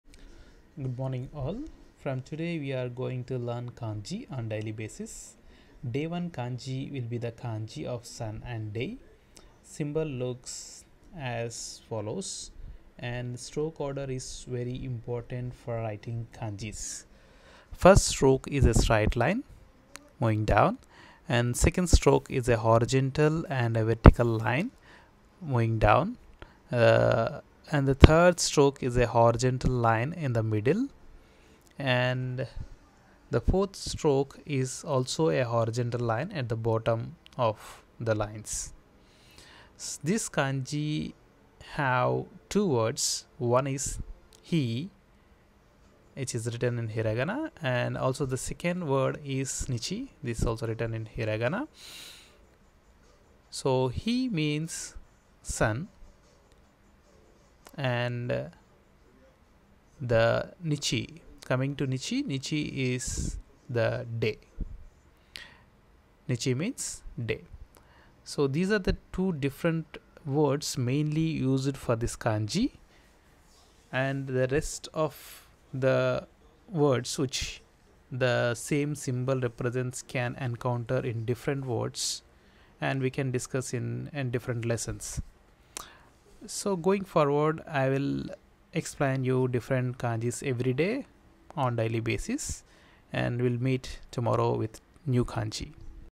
Daily kanji Day 1 -N5 in English
Learn one kanji everyday and you can easily pass the JLPT exam. Now you can get daily one kanji in easy way following this series and keep watching @jlpt